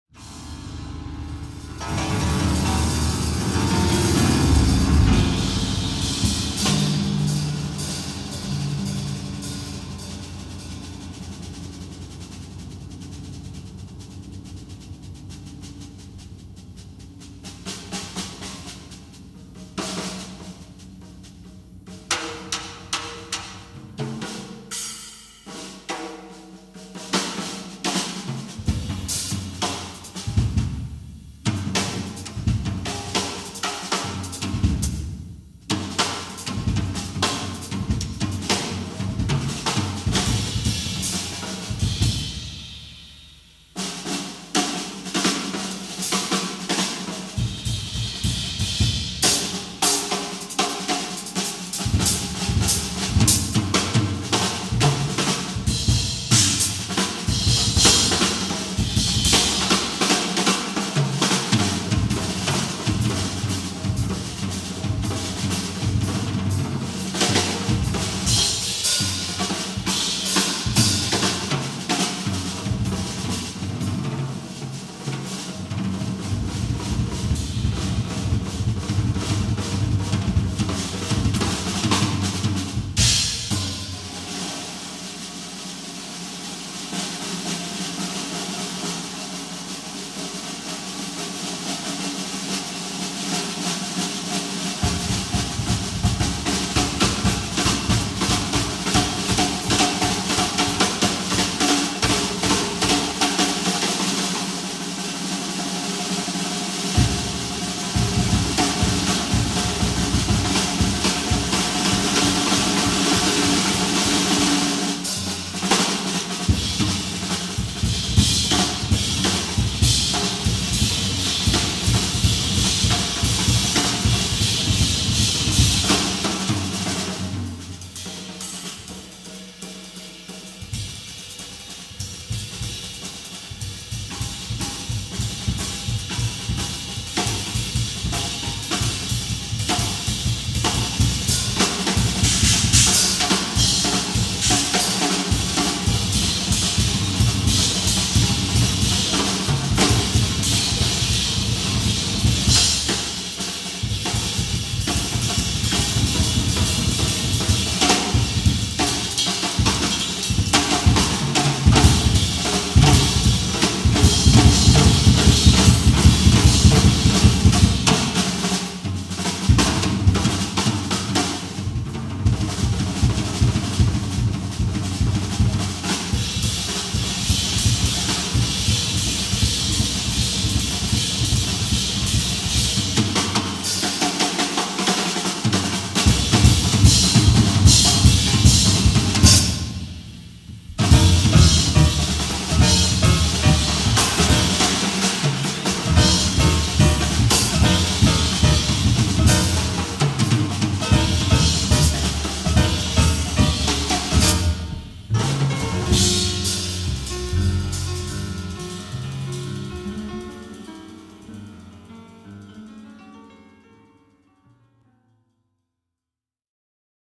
Drum bridge-NUKeLEUS
An unusually mellow drum solo of mine from the Novi Sad concert.
(The  Nile wristbands were given to me by GK and I always wear them for good luck!)